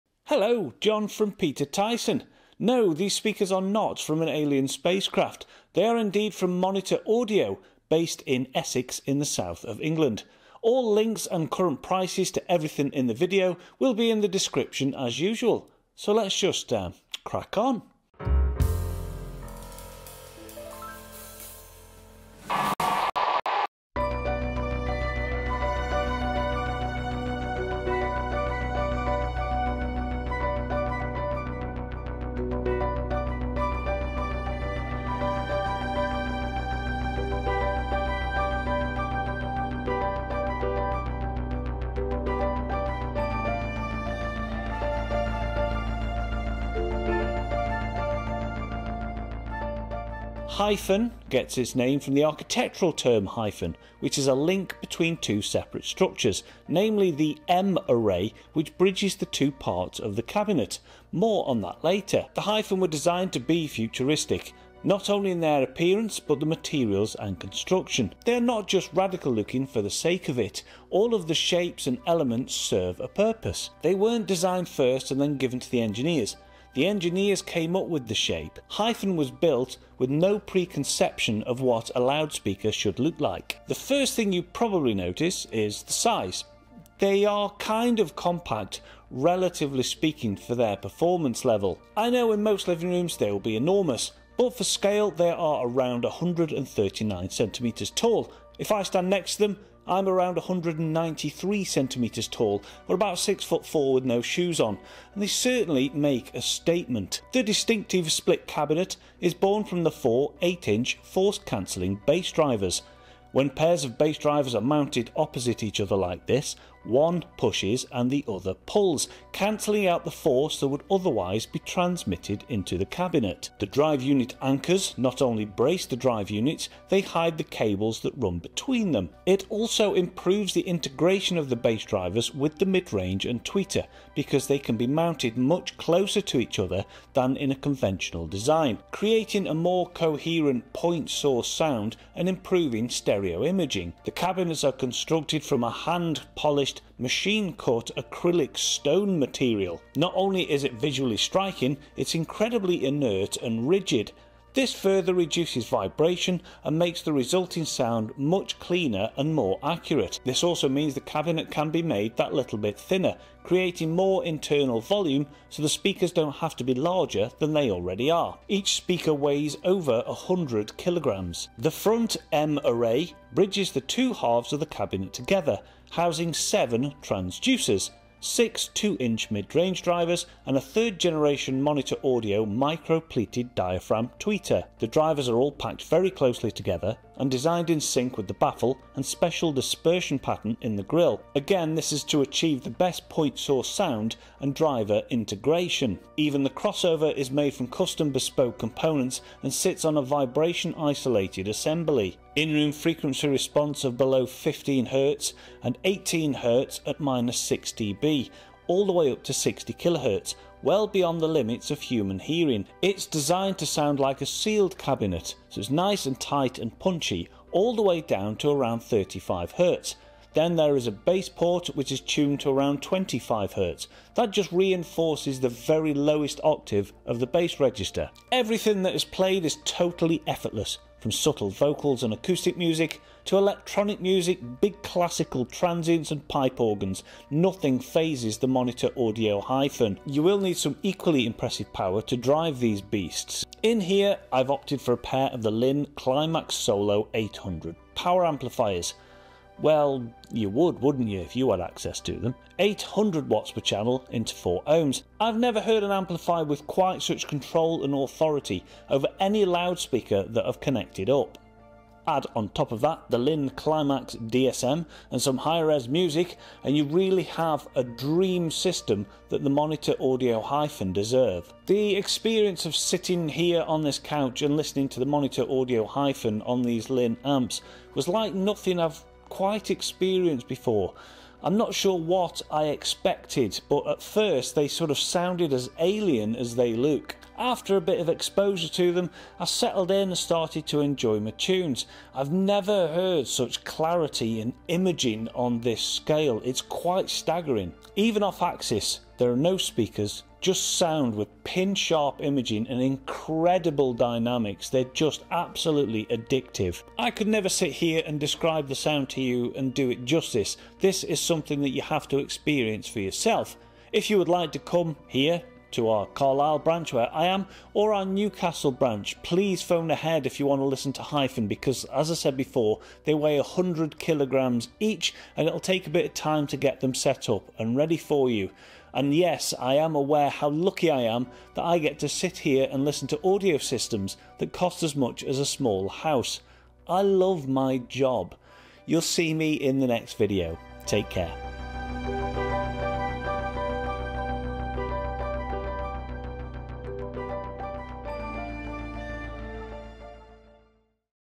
Monitor Audio Hyphn : Flagship Speakers Like No Other
The new futuristic, flagship, 50th anniversary speakers from Monitor Audio.

*PT VIP* - Sign up for our latest promotions, VIP prices, and monthly prize draw.

Other products in the video...

 CHAPTERS
0:00 - Intro
0:21 - Overview
1:47 - Sound & Technical Details
3:48 - Test System
4:31 - John's Thoughts